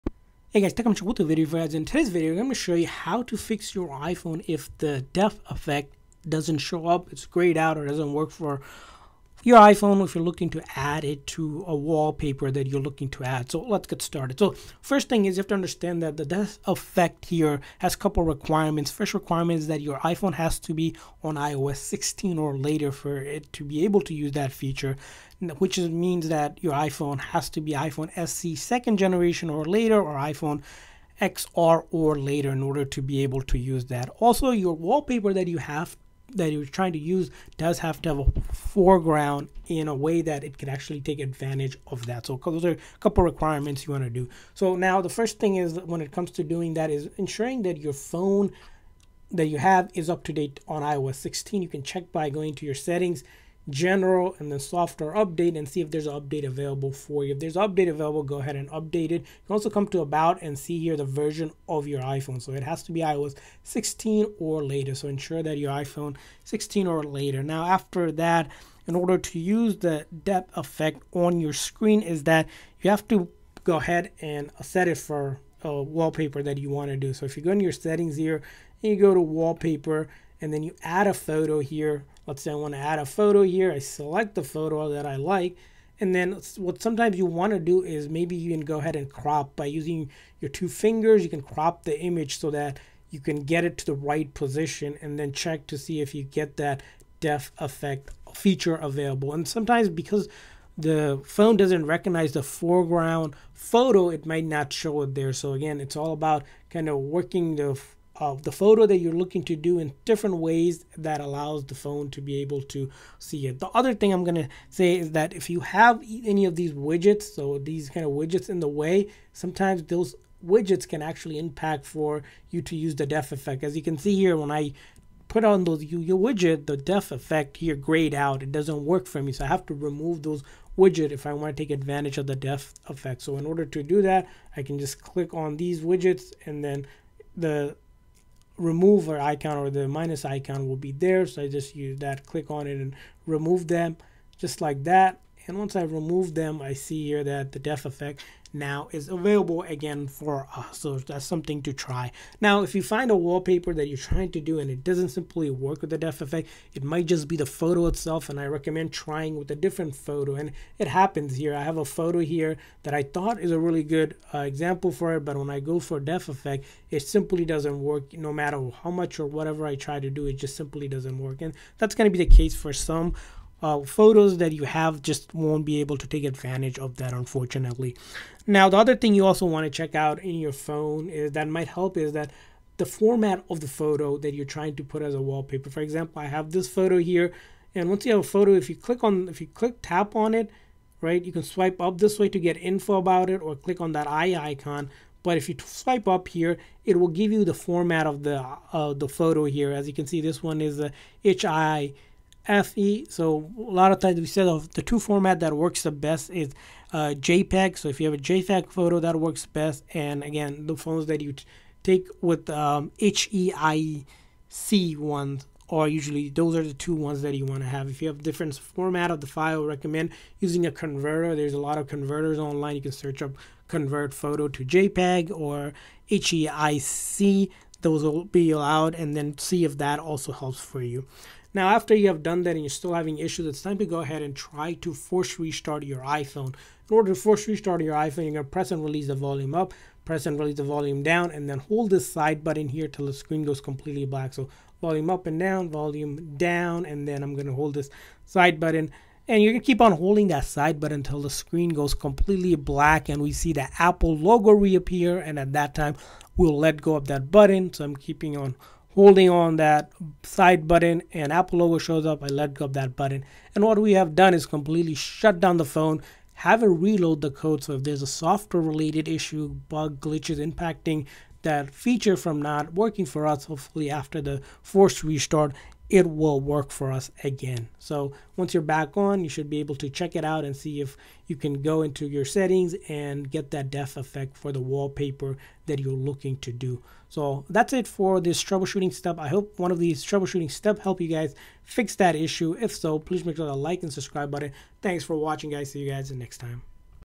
How to Fix Depth Effect on Wallpaper Not Working on iPhone
Here's how to fix the depth effect not working or grayed out on wallpaper for your iPhone. Depth effect is new feature part of iOS 16 but sometimes it doesn't work. However you can fix the problem by following these step by step guide.